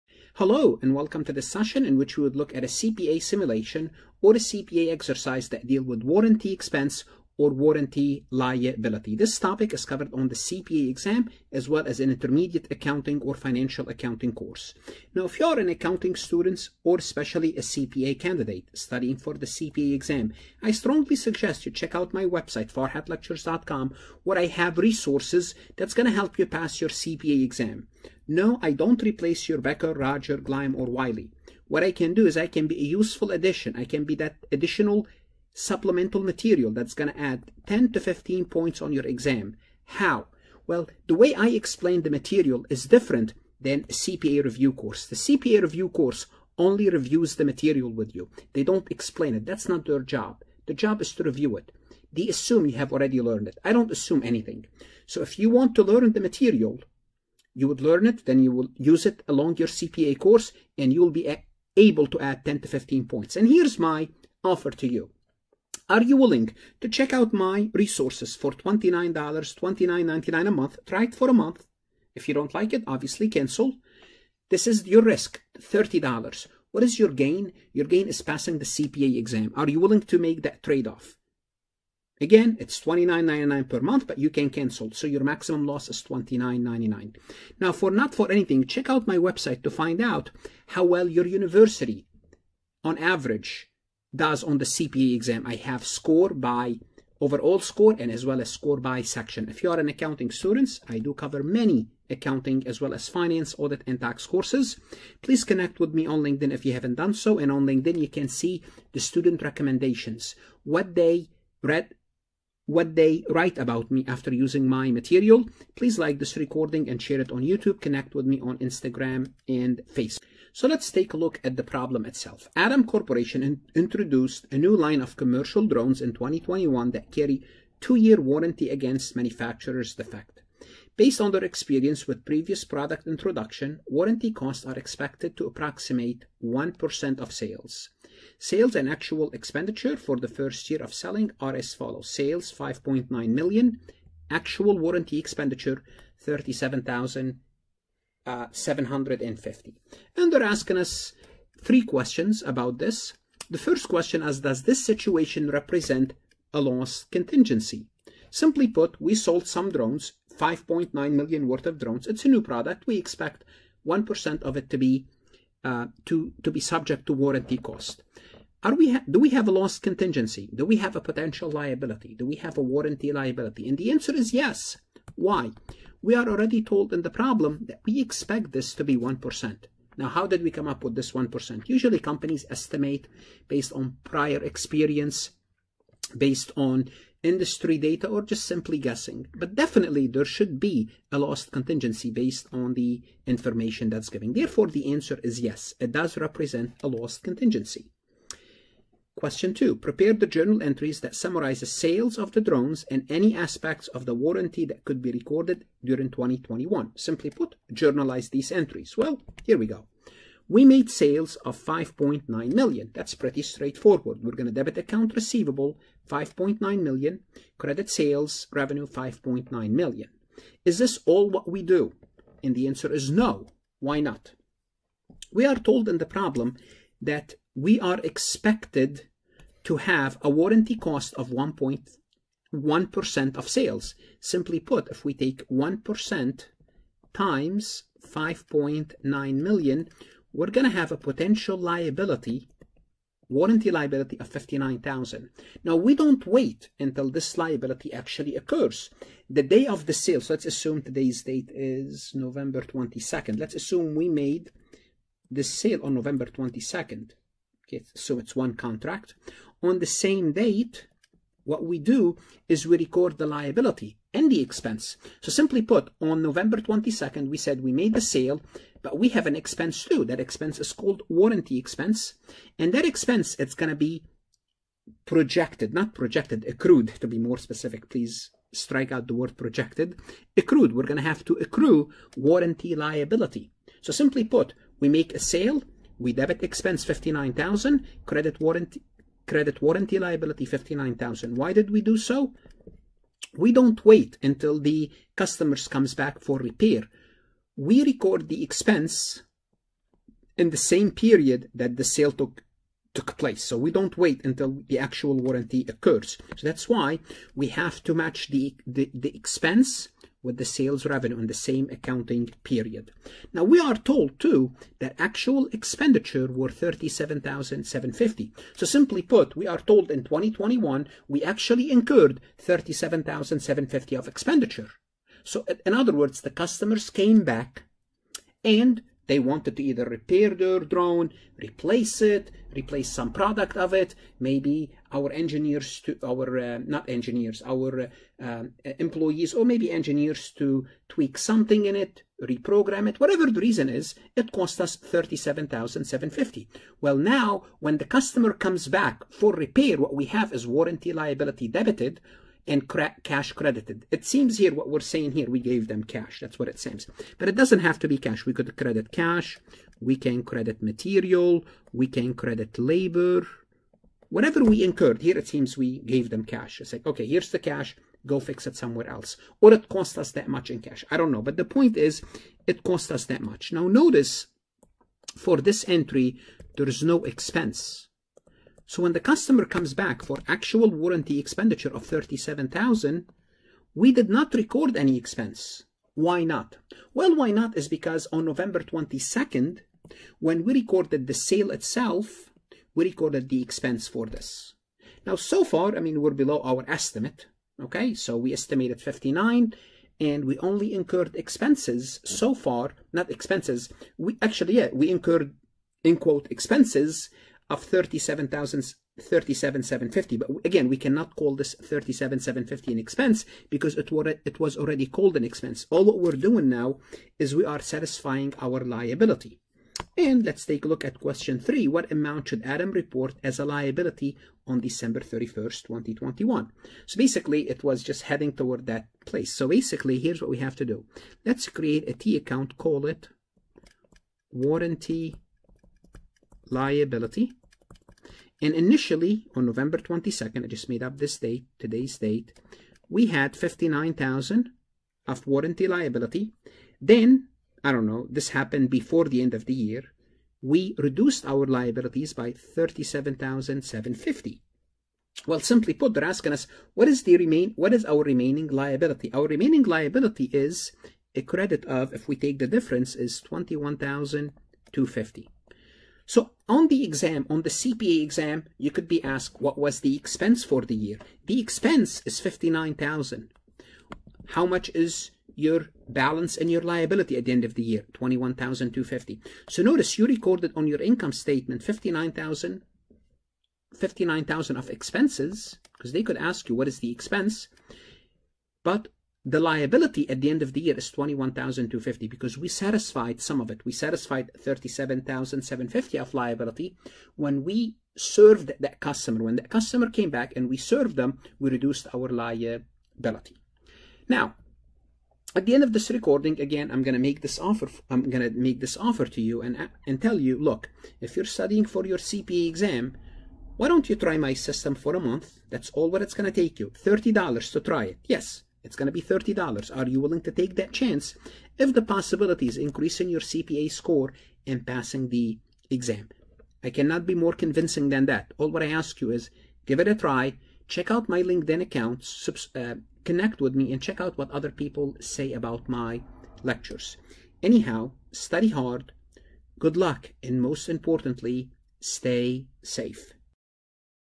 CPA Exam Simulation -Warranty Expense/Obligation
In this session, I cover how to answer CPA exam simulation about warranty expense or warranty obligation.  CPA Task-Based exam Simulation is a scenario-based questions on the CPA Exam. CPA exam simulation is a type of questions on the CPA exam. Farhat Accounting lectures can help you prepare to answer CPA exam simulation on the exam day.